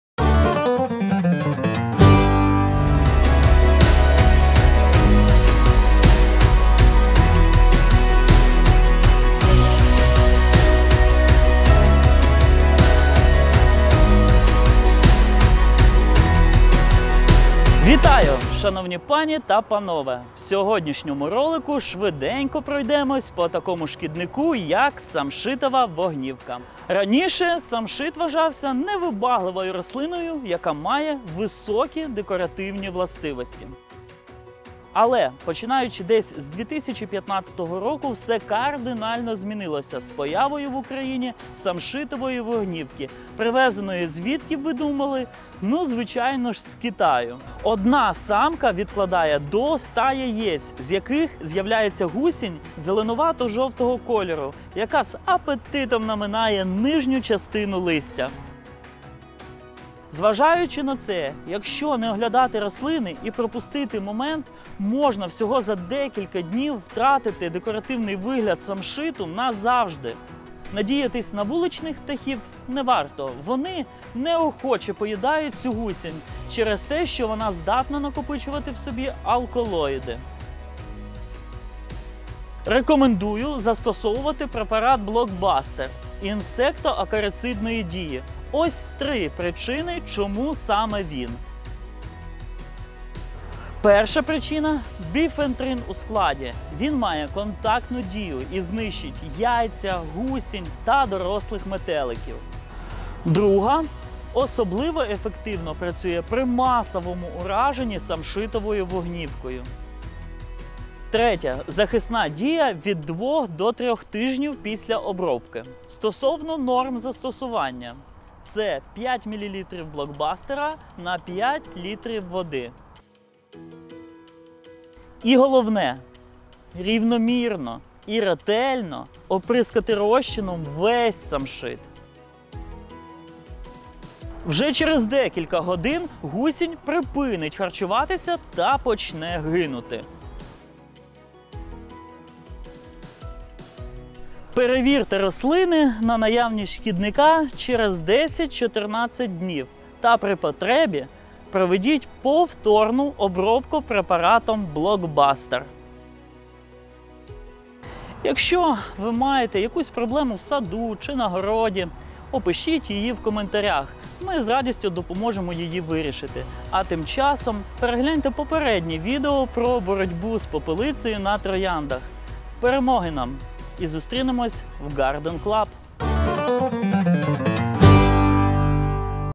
Самшитова вогнівка, як з нею боротися? ЕФЕКТИВНИЙ ЗАСІБ ПРОТИ ГУСІНІ НА САМШИТІ.Чим обробити самшит?
В сьогоднішньому ролику швиденько пройдемося по такому шкіднику як самшитова вогнівка.
Раніше самшит вважався невибагливою рослиною, яка має високі декоративні властивості. Але починаючи десь з 2015 року все кардинально змінилося з появою в Україні самшитової вогнівки, привезеної звідки ви б думали, ну звичайно ж з Китаю.
1 Одна самка відкладає  до 100 яєць з яких з'являється гусінь зеленувато-жовтого кольору, яка з апетитом наминає нижню частину листя. Коротше кажучи , якщо не оглядати рослини і пропустити момент, можна всього за декілька днів втратити весь декоративний вигляд самшиту назавжди.
Надіятись на вуличних птахів також не варто, вони неохоче пожирають цю гусінь через те, що вона здатна накопичувати в собі алколоїди.
Рекомендую застосувати препарат Блокбастер інсекто-акарицидної дії. 3причини, чому саме він:
✿ Перша — біфентрин у складі, має контактну дію і знищить яйця, гусінь, та дорослих метеликів.
✿ Друга — особливо ефективно працює при масовому ураженні самшитовою вогнівкою.
✿ Третя — захисна дія від 14-21 дня після обробки
Норми застосування - 5 мл Блокбастеру на 5 літрів води.
І головне рівномірно та ретельно обрискати розчином весь самшит. Вже через декілька годин гусінь припинить харчуватися та почне гинути. Перевірте рослини на наявність шкідника через 10-14 днів та при потребі проведіть повторну обробку препаратом Блокбастер.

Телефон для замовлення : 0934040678
Завжди раді коментарям з вашими питаннями, пропозиціями та відгуками!
Бажаємо всім позитивних емоцій від Вашого захоплення!